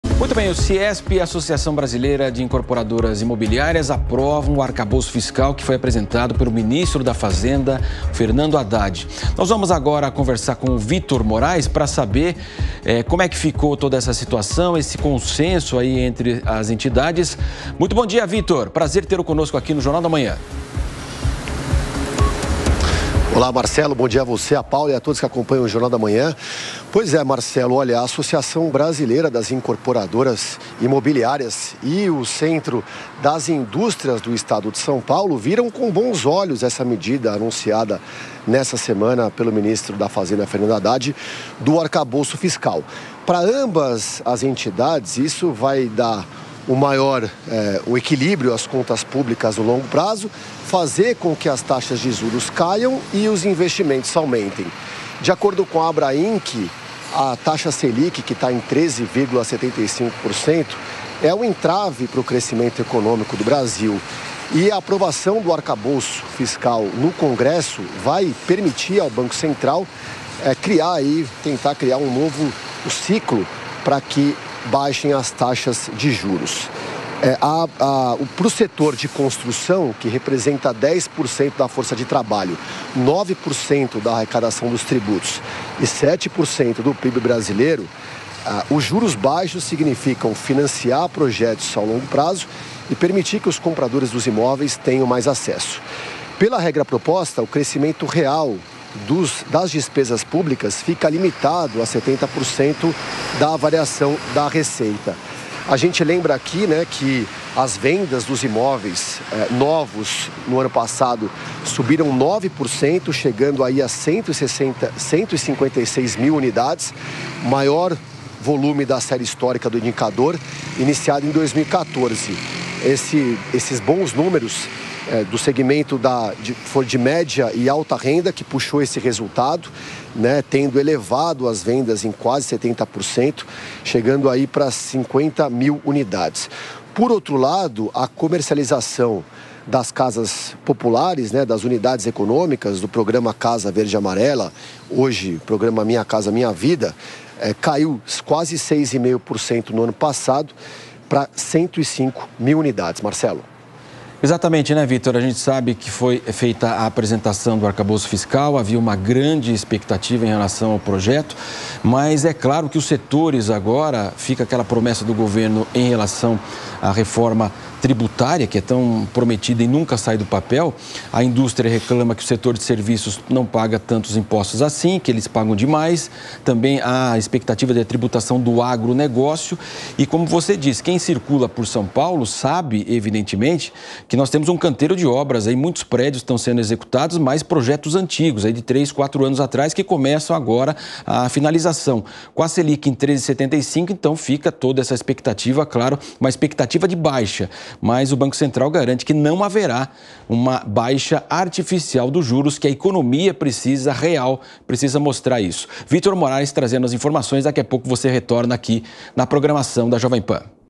Novo arcabouço fiscal é aprovado pelo Ciesp e Associação Brasileira de Incorporadoras Imobiliárias
O novo arcabouço fiscal, apresentado pelo governo Lula (PT), foi aprovado por duas entidades da economia: o Ciesp (Centro das Indústrias de São Paulo) e a Associação Brasileira de Incorporadoras Imobiliárias.